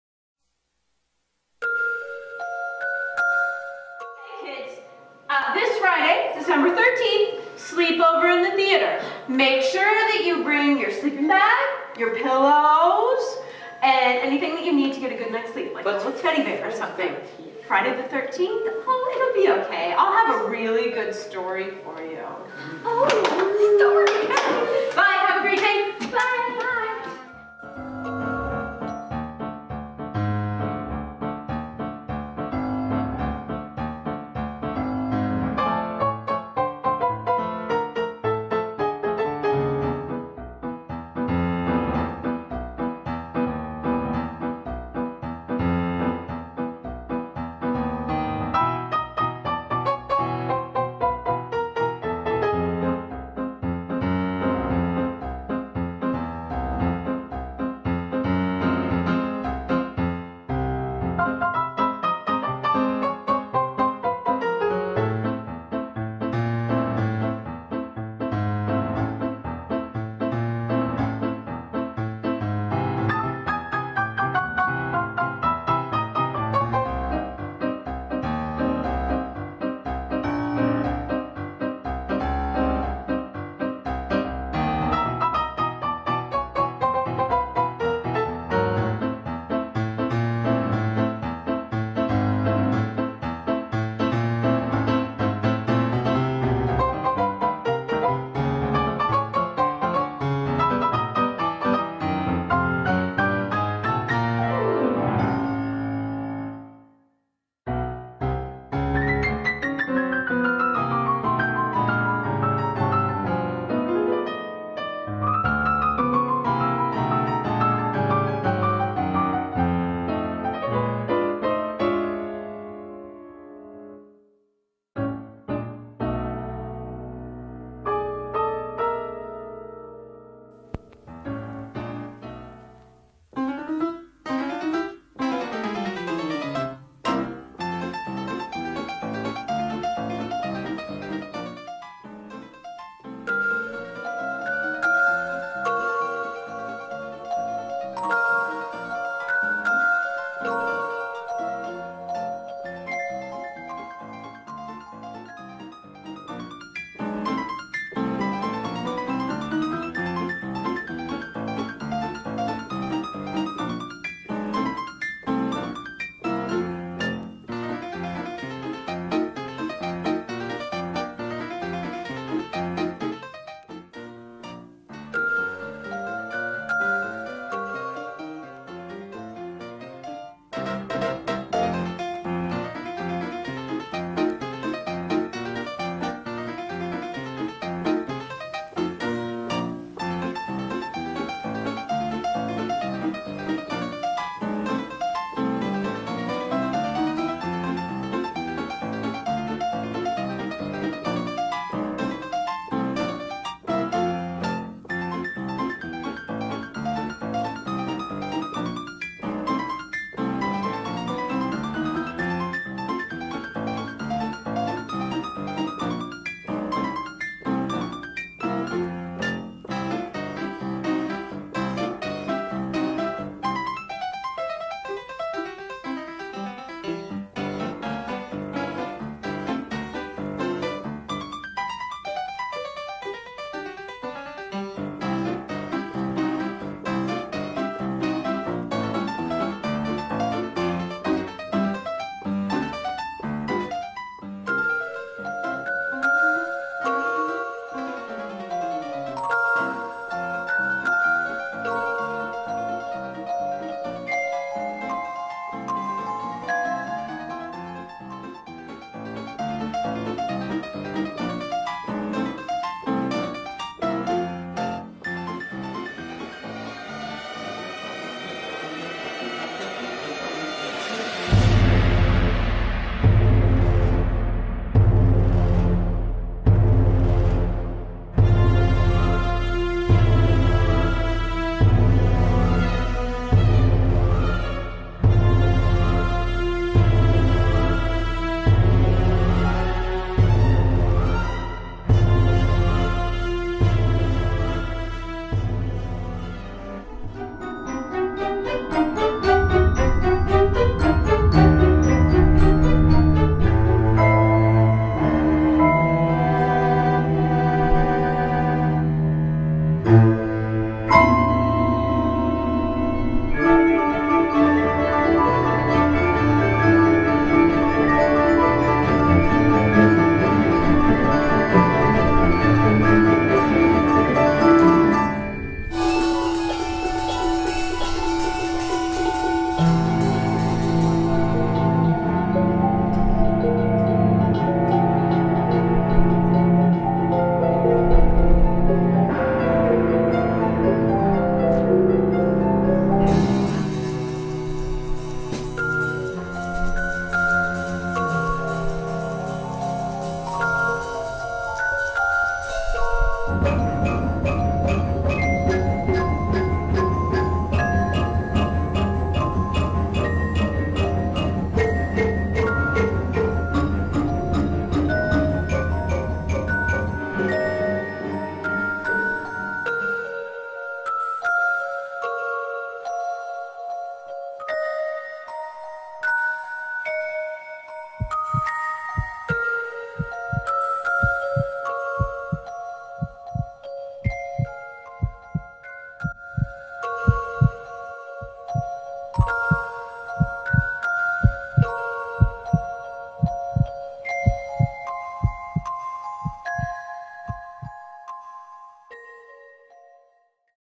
Ghost in the Theater
The 6th graders made a silent movie for their final in Drama this semester.  The students came up with the idea, wrote the script, and we filmed the video together the last week of classes.  Although it might be more appropriate as a Halloween story, I hope you enjoy their creativity as much as I have all semester long!